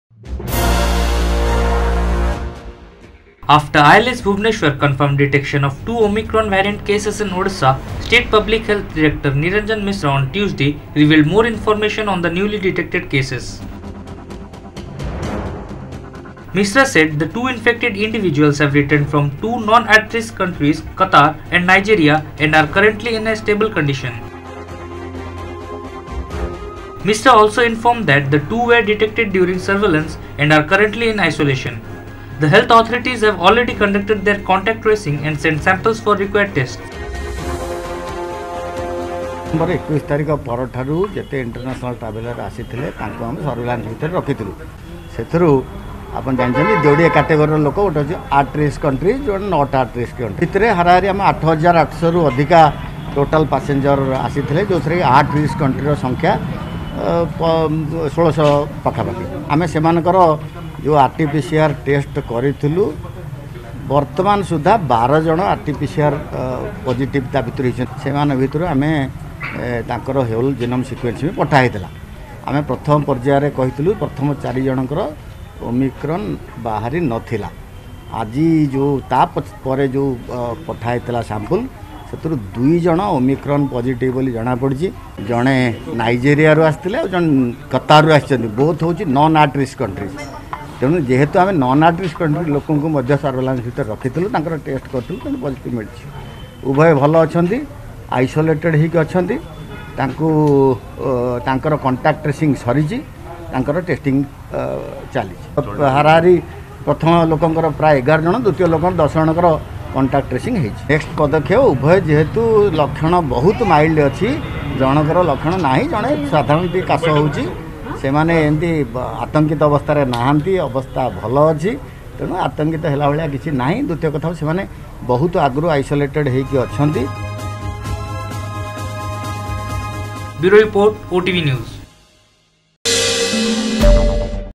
Omicron In Odisha: 2 Infected From Non At-Risk Counties, Have Mild Symptoms: Top Health Official
After ILS (Institute of Life Sciences) Bhubaneswar confirmed detection of two ‘Omicron’ variant cases in Odisha, state Public Health Director, Niranjan Mishra on Tuesday (Dec 21) revealed more information on the newly detected cases.